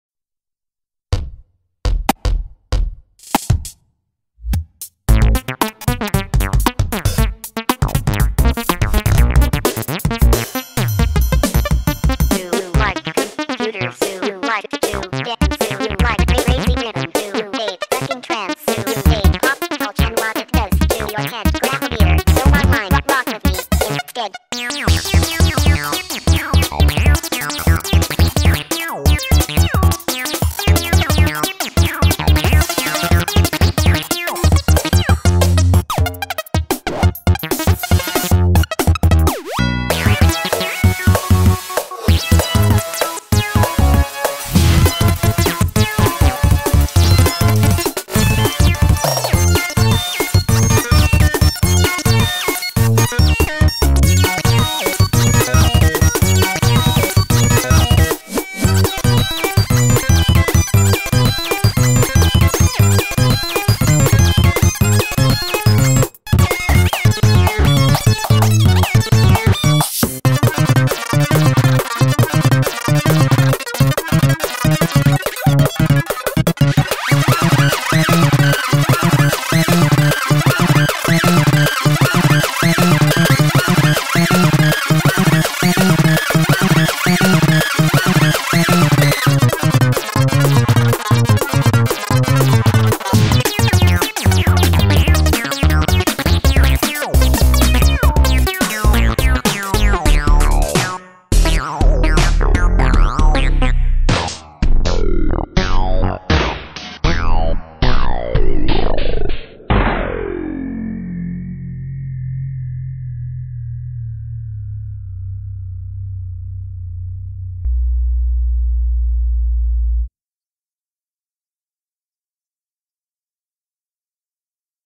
Tigers 8 Bit
I took a picture with my Gameboy Camera and used Ed Halley's buildup script to reconstruct this pixel by pixel.